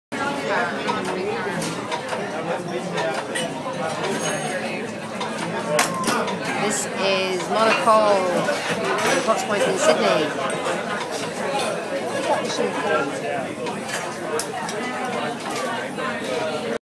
Monopole Champagne Bar - Mean Machine Meat Slicer - Sydney
Champagne Jayne pops into new Potts Point eaterie & wine bar Monopole by Brent Savage & Nick Hildebrandt to check out Sydney's latest mean meat machine & champagne joint.